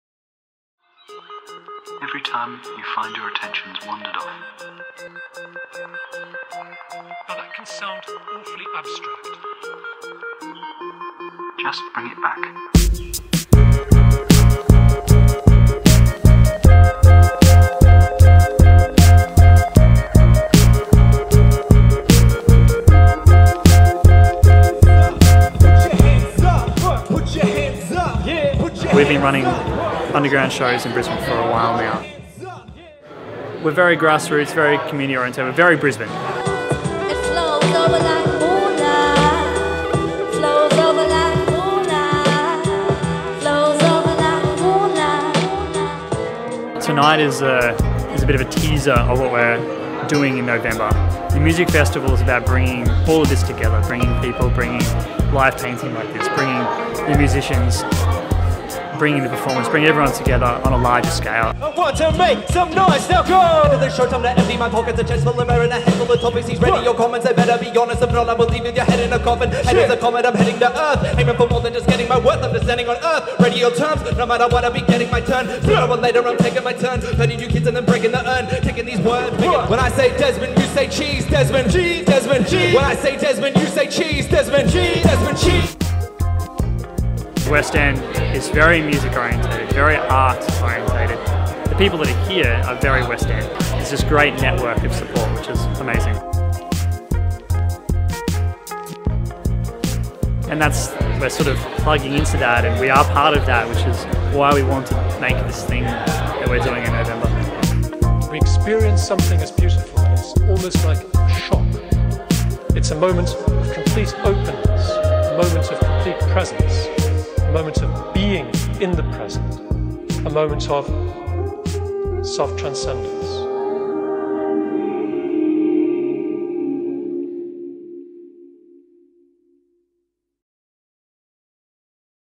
Jungle Love Festival Launch @ The Motor Room in West End
See it first on 4101TV. Launching the very first Jungle Love Festival @ The Motor Room in Brisbane's West End on Saturday night. Just a taster of the vibe to be expected at the festival in November.

Additional Photographs by Sam Navin
Artist: Blu Art Xinja, Barek Art & Cherie Strong